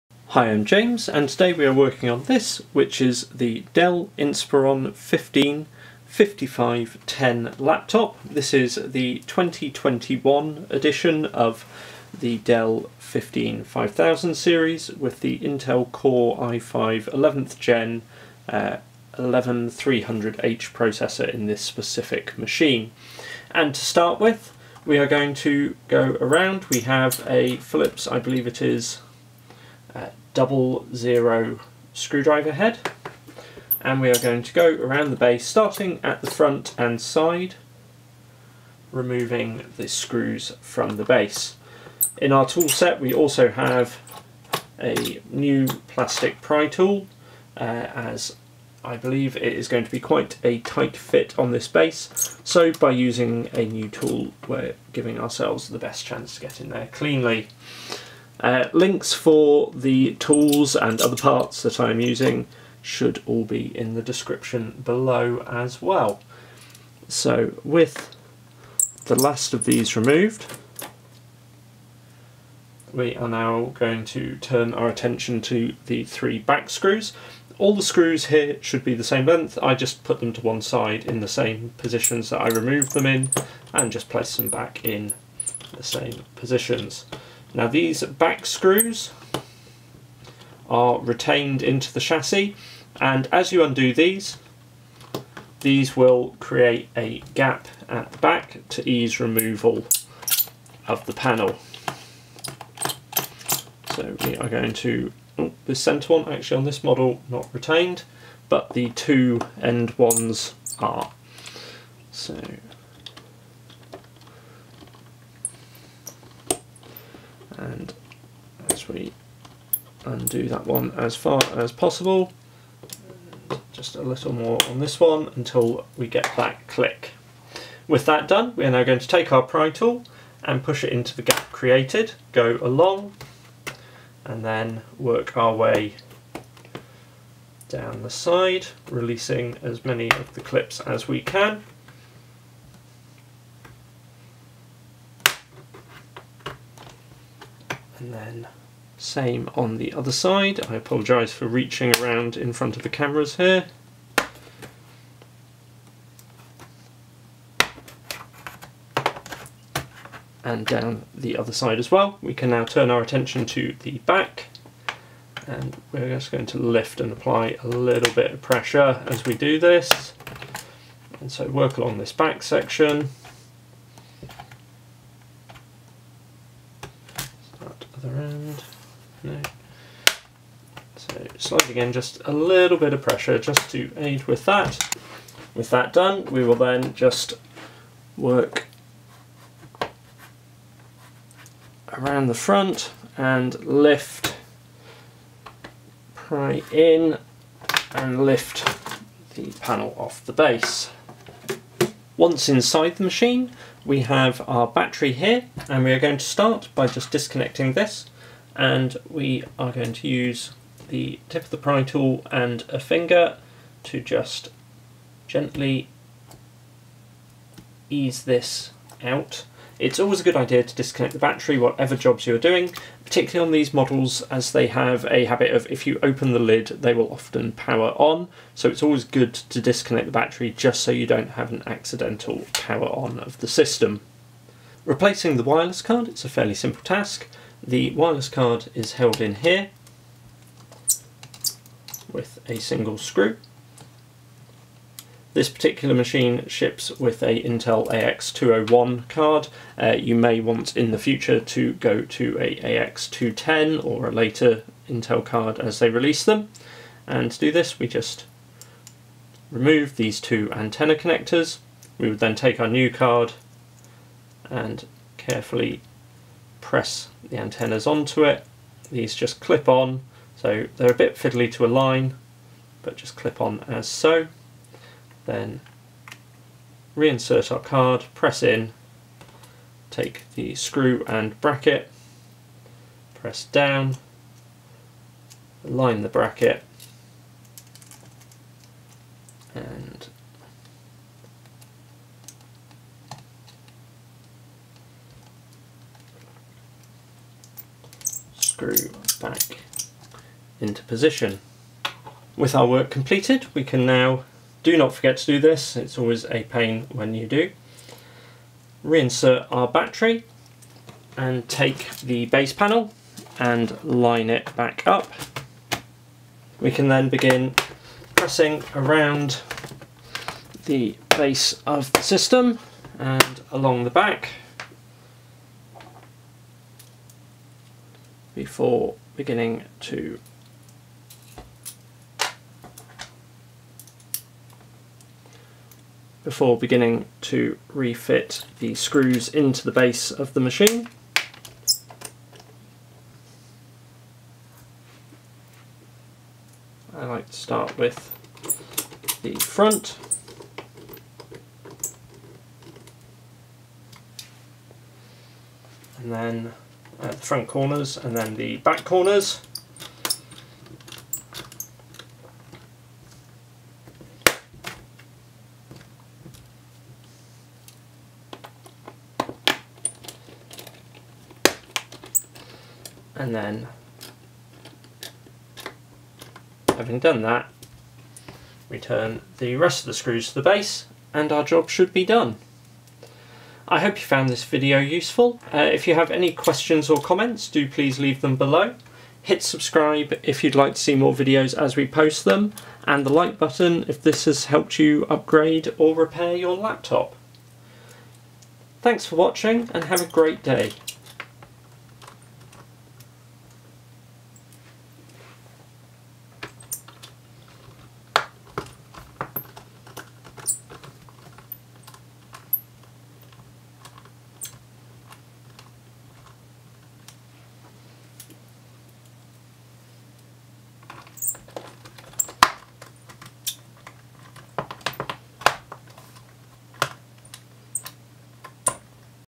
Dell Inspiron 15-5510/5518 - M.2 WiFi Card Replacement Guide
A guide to replacing the M.2 WiFi Card of the Dell Inspiron 15-5510 laptop.

This is a relatively simple process requiring just a small Philips head screwdriver and a plastic pry tool.

Screwdriver set
Pry tools

0:00 Base removal
3:22 Battery Disconnection and Removal
3:53 M.2 WiFi Card Replacement
5:02 Battery and Base Refitting